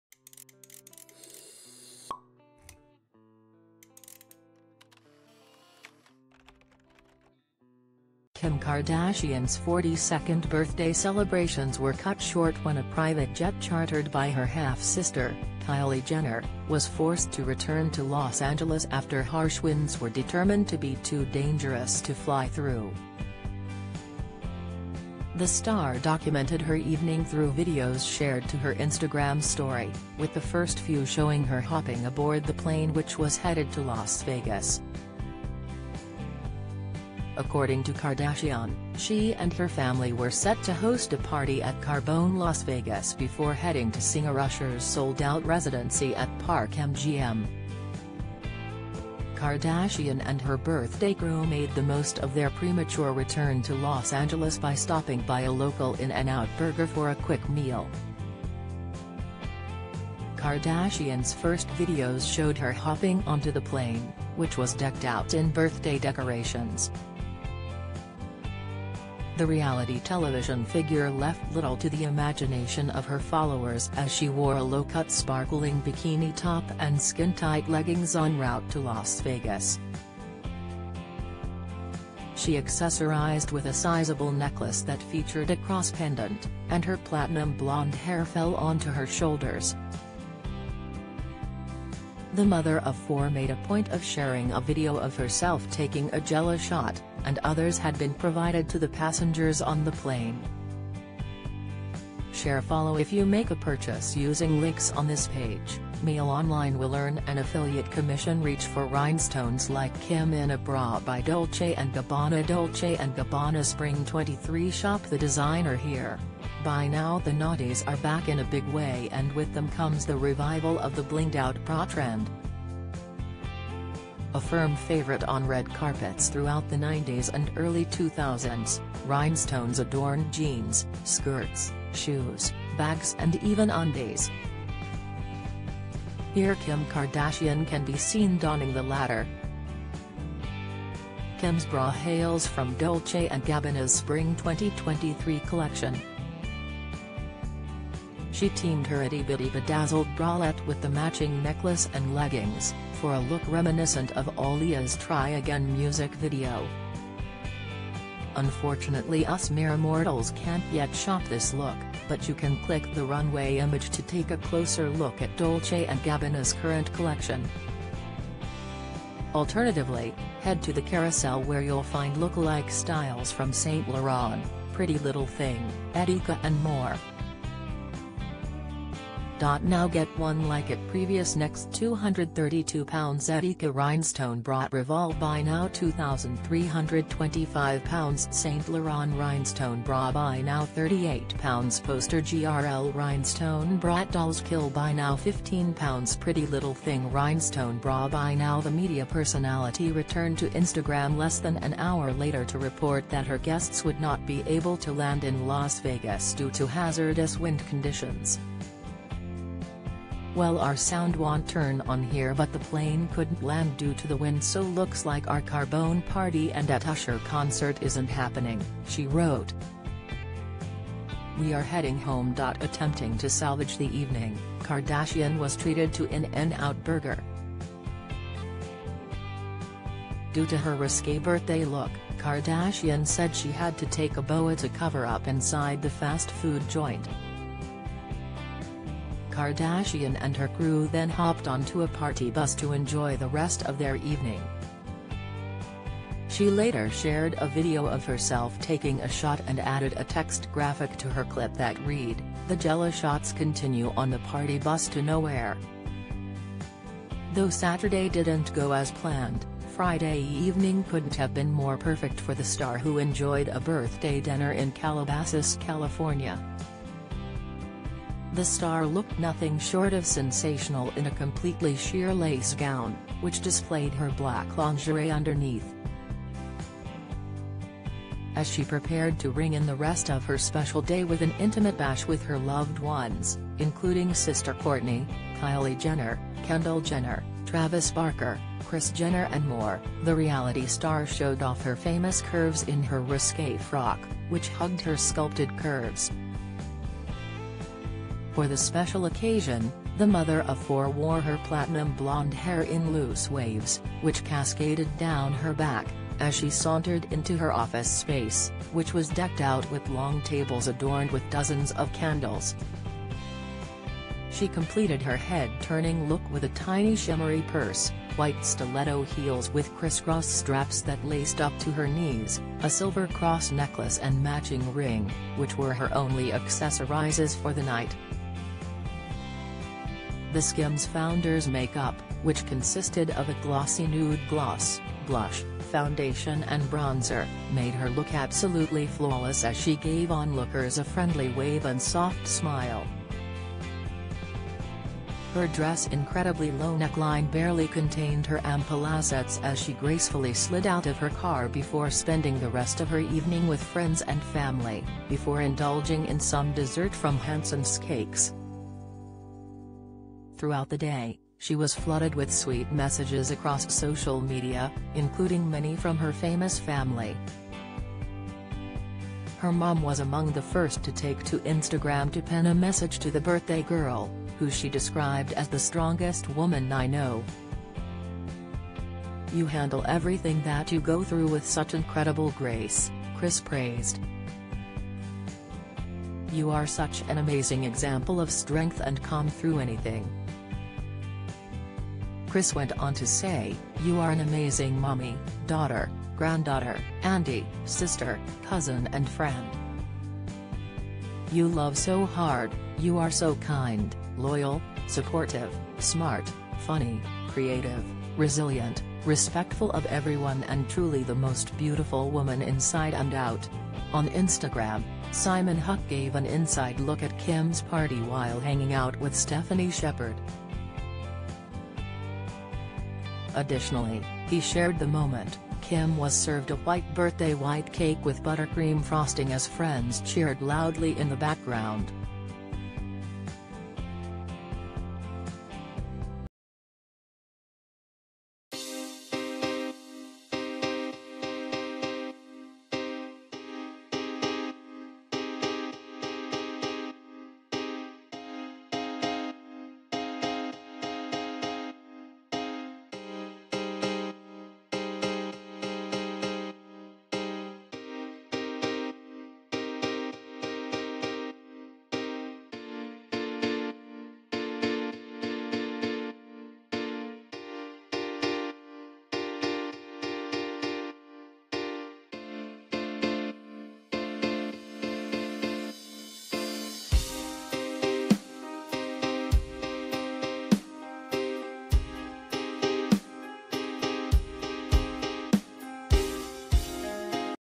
Kim Kardashian's 42nd birthday celebrations are cut short as sister Kylie Jenner's jet returns to LA
Kim Kardashian's 42nd birthday celebrations are cut short as sister Kylie Jenner's jet returns to LA 345259